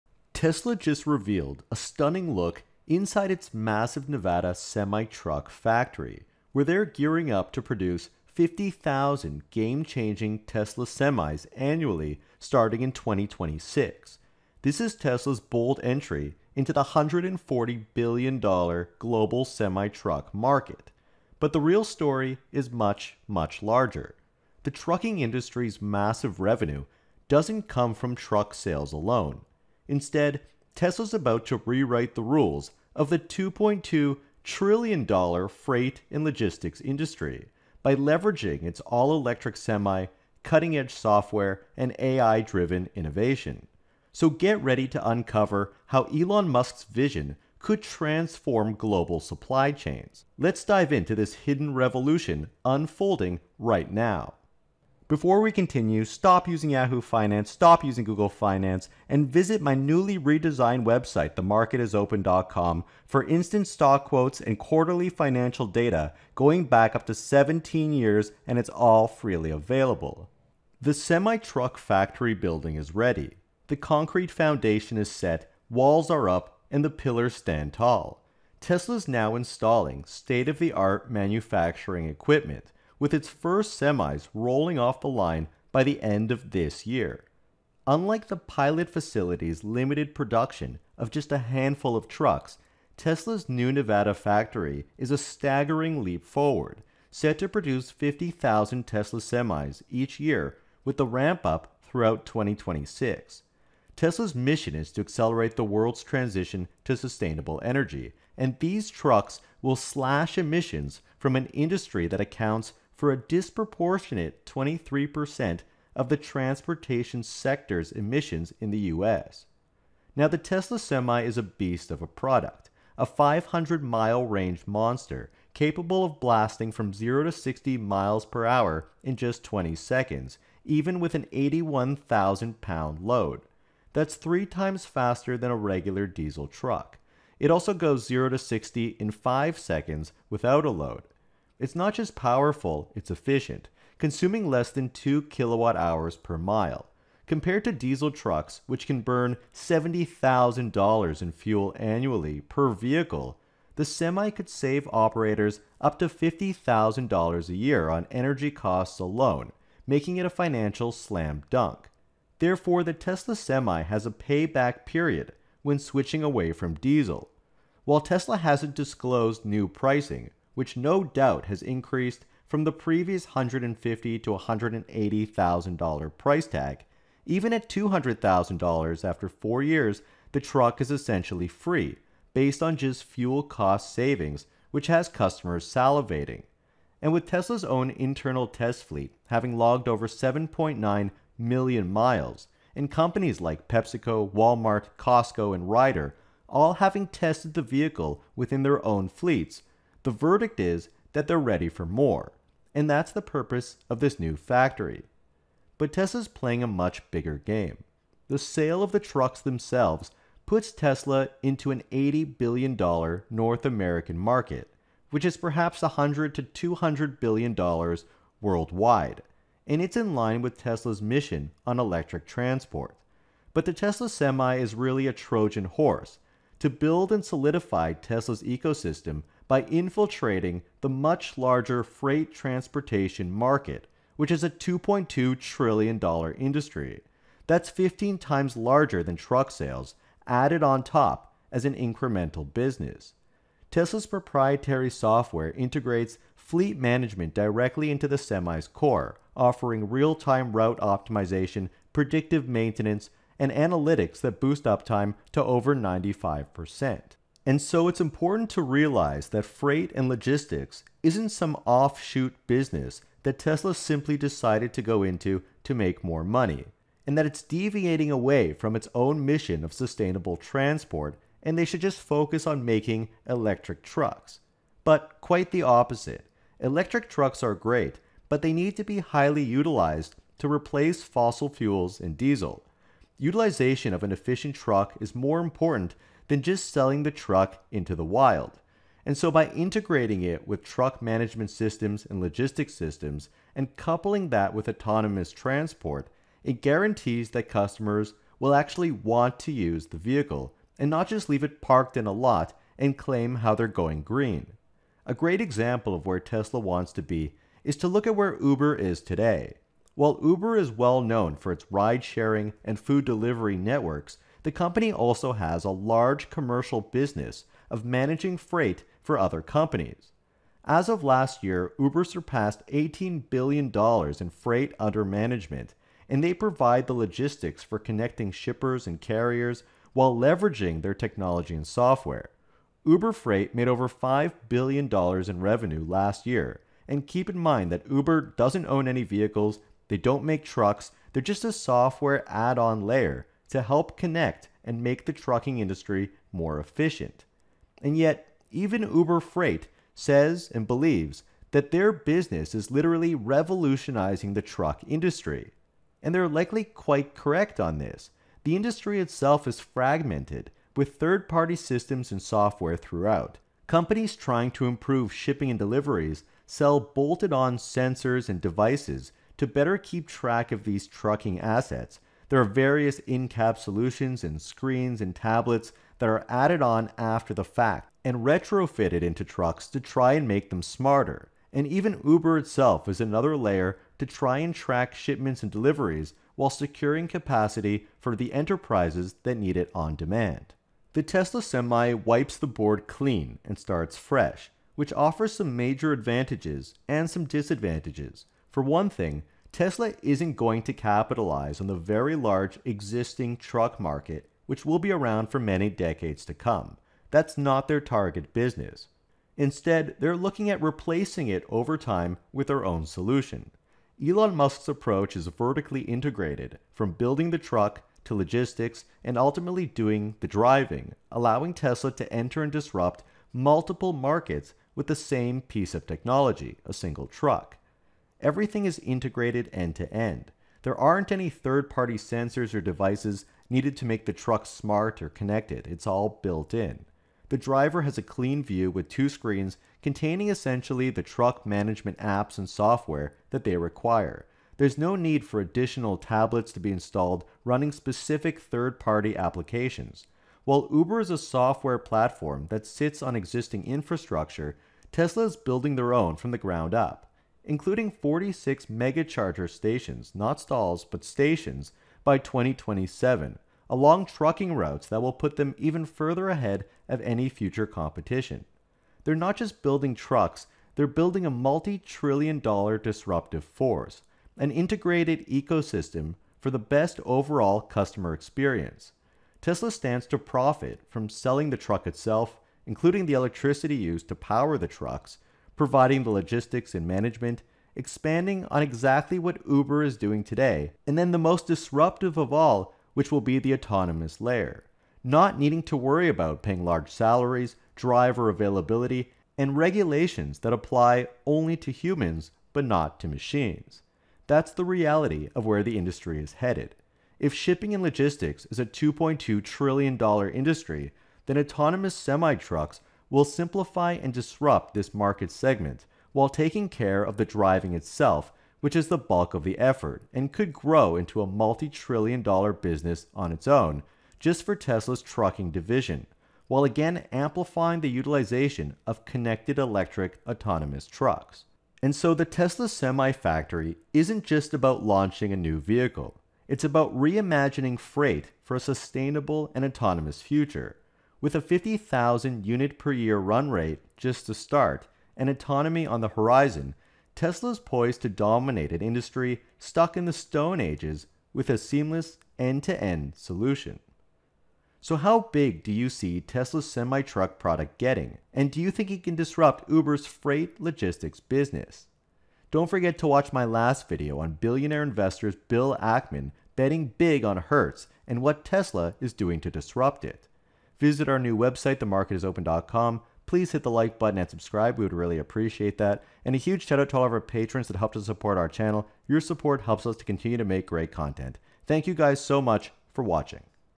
Tesla's New Semi Factory is Building MORE Than Trucks! ($2.2 TRILLION SECRET)
Tesla's new Semi Truck factory is just the beginning. Discover how Elon Musk is taking on the $2.2 TRILLION dollar freight industry with a vertically integrated approach, aiming to be the Uber of trucking but owning the entire stack – the electric trucks, the AI logistics, and eventually, the autonomous driving. This is a disruption on a scale you haven't seen.

🎥 The REAL Reason Tesla is Firing Up its Lithium Refinery (And It's NOT What You Think)

👕 Elon Musk Innovation
👕 e ln(musk) Integral T-Shirt